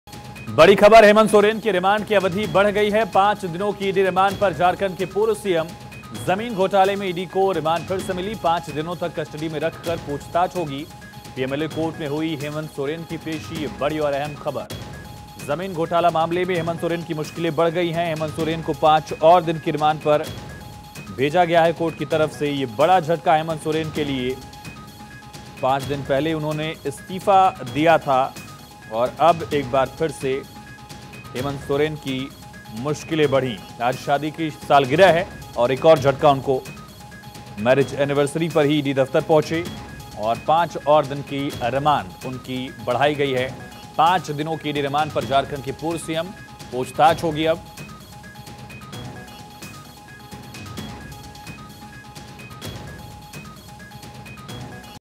Hemant Soren ED Remand : हेमंत सोरेन की बढ़ी मुश्किलें, 5 दिन की बढ़ी रिमांड | Jharkhand News
जमीन घोटाले में गिरफ्तार राज्य के पूर्व मुख्यमंत्री हेमंत सोरेन को पीएमएलए कोर्ट में फिर से बुधवार को पेश किया गया। बड़गाई अंचल के 8.50 एकड़ जमीन घोटाले मामले में गिरफ्तार हेमंत सोरेन के पांच दिन की रिमांड 7 फरवरी हो खत्म हुई। ईडी ने हेमंत सोरेन से पूछताछ के लिए अपनी रिमांड अवधि 7 दिन और बढ़ाने की मांग कोर्ट से की। लेकिन अदालत ने सिर्फ पांच दिन और रिमांड की अनुमति दी गई।\

Former Chief Minister of the state Hemant Soren, arrested in the land scam, was produced again in the PMLA court on Wednesday. The five-day remand of Hemant Soren, arrested in the 8.50 acre land scam case of Badgai area, ended on 7 February. ED demanded from the court to extend its remand period by 7 more days for the interrogation of Hemant Soren. But the court allowed remand for only five more days.

Anchor Name : Ayush Suryavanshi | Show Name : News | Time : 05: 00 PM | Date : 07/02/2024 | Published By : Mansi